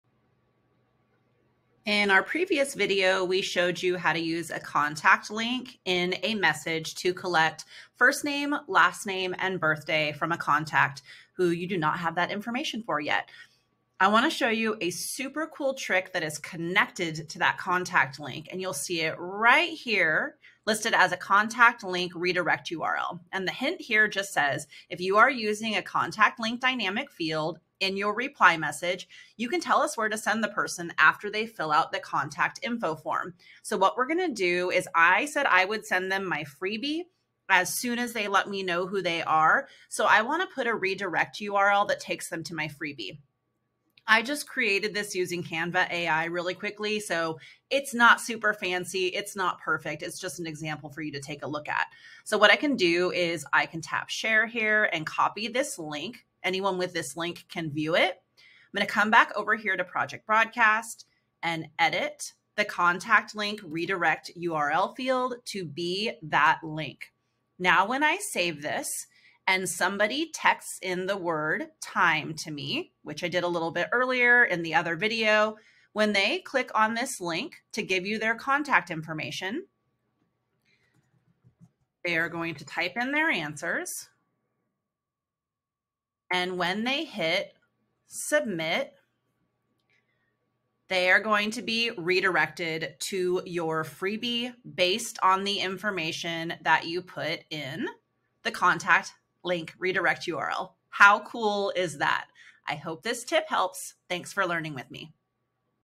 What is a Contact Link Redirect url?
Have you gathered a new contact's info with our contact link dynamic field? Want them to be automatically redirected to a freebie, booking link, or any other url once they give you their info? You've gotta check this trick out!

Want to learn more? Our Web School course on dynamic fields is
Complete the course and we'll add 10 credits to your account!